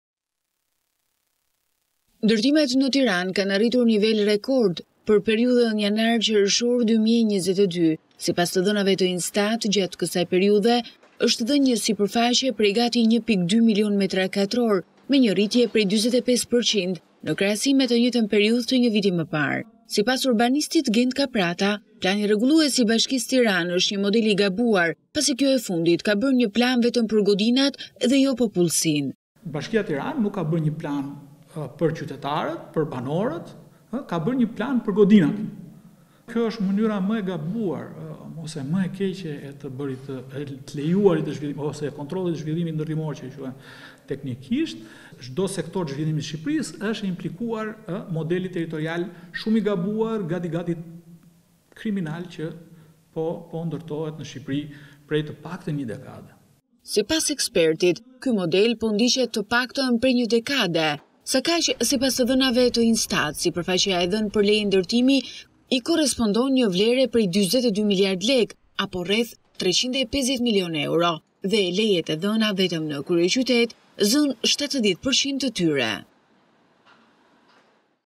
Rekord në Tiranë/ Në 6 muaj u dhanë 1.2 milionë metra katror leje ndërtimi
Sipërfaqja që është dhënë për leje ndërtimi në Tiranë ka arritur një nivel rekord në 6 muajt e parë të këtij viti.

Sipas të dhënave të INSTAT, në kryeqytet, për periudhën janar- qershor 2022 është dhënë një sipërfaqe prej gati 1.2 milionë metra katror, me një rritje prej 45% në krahasim me të njëjtën periudhë të një viti më parë.

Sipas urbanistit, Gent Kaprata, plani rregullues i bashkisë Tiranë është një model i gabuar pasi kjo e fundit ka bërë një plan vetëm për godinat dhe jo popullsinë.

"Bashkia Tiranë nuk ka bërë një plan për qytetarët, për banorët por ka bërë një plan për godinat. Kjo është mënyra më e gabuar ose më e keqe e kontrollit të zhvillimit ndërtimor. Në çdo sektor të zhvillimit të Shqipërisë është implikuar modeli territorial shumë i gabuar, gati-gati kriminal që po ndërtohet në Shqipëri prej të paktën një dekade."

Sipas ekspertit, ky model po ndiqet prej të paktën një dekade. Sakaq sipas INSTAT, sipërfaqja e dhënë për leje ndërtimi i korrespondon një vlere prej 42 mld lekë apo rreth 530 mln euro.

Lejet e dhëna në kryeqytet zënë 70% të totalit të tyre në gjithë territorin.